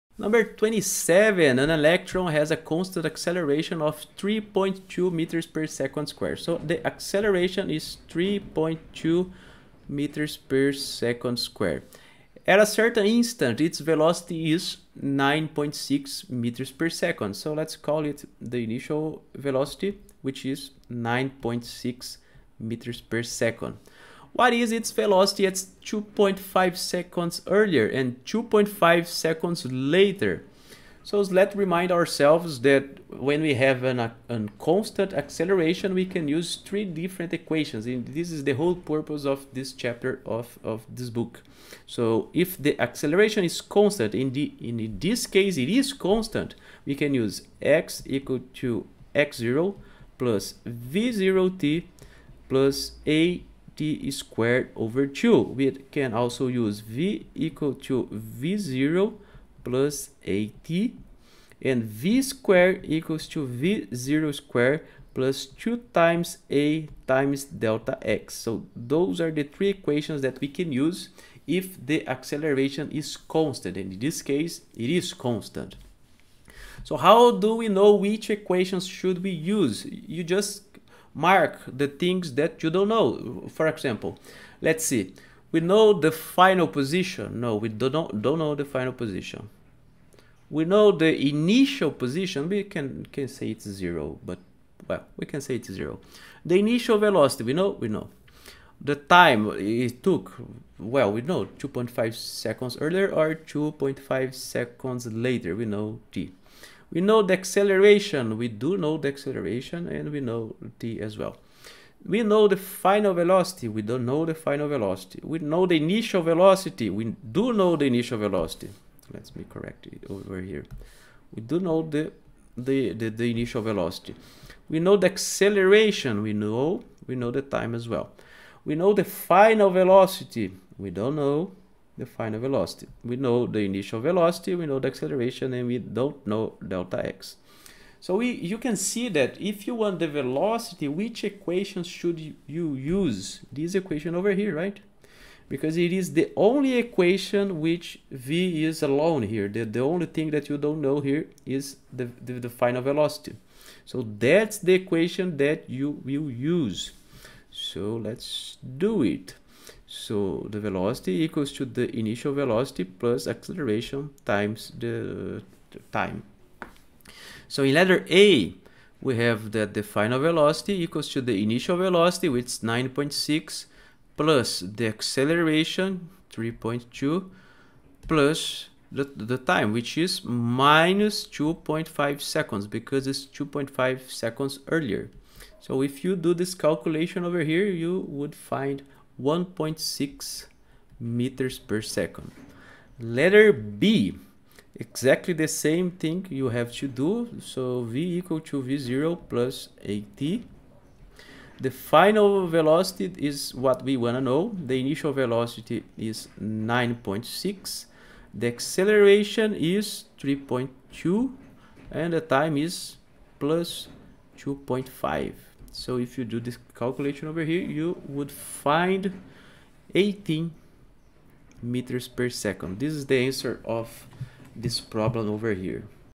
HALLIDAY SOLUTIONS - CHAPTER 2 PROBLEM 27 - Fundamentals of Physics 10th
An electron has a constant acceleration of 3.2 m/s2. At a certain instant its velocity is 9.6 m/s.What is its velocity (a) 2.5 s earlier and (b) 2.5 s later?